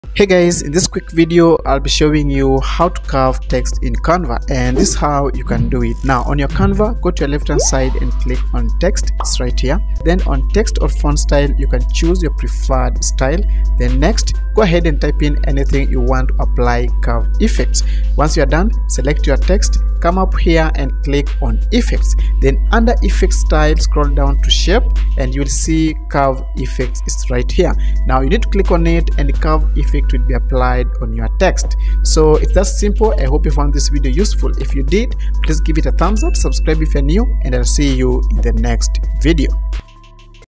How to Curve Text in Canva | Canva Tutorial
In this video, I'll show you exactly how to curve text in Canva, allowing you to add a unique touch to your creations. With step-by-step instructions, you'll learn how to bend text in Canva effortlessly, creating beautifully curved text effects that will make your designs stand out.